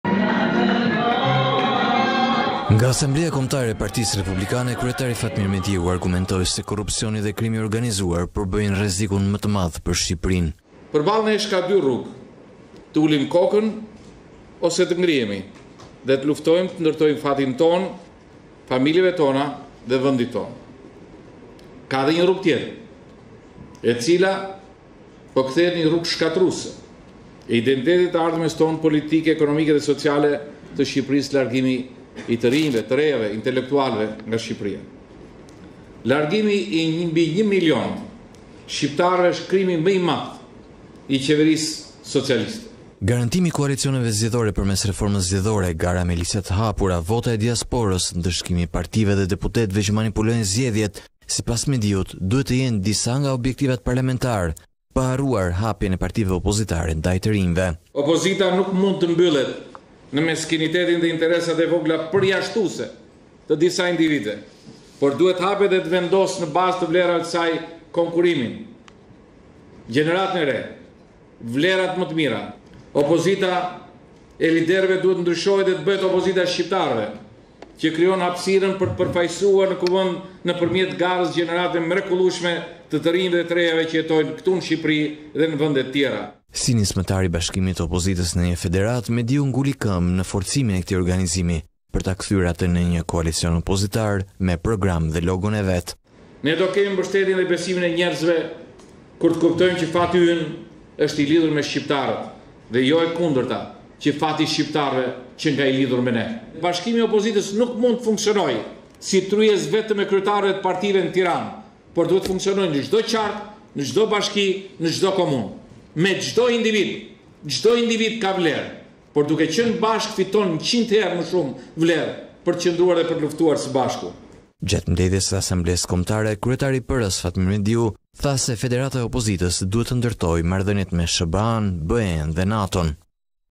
Asambleja Kombëtare e PR, Mediu: Bashkimi i opozitës, e vetmja rrugë përballë Ramës
Nga Asambleja Kombëtare e Partisë Republikane, kryetari Fatmir Mediu argumentoi se korrupsioni dhe krimi i organizuar përbëjnë rrezikun më të madh për Shqipërinë.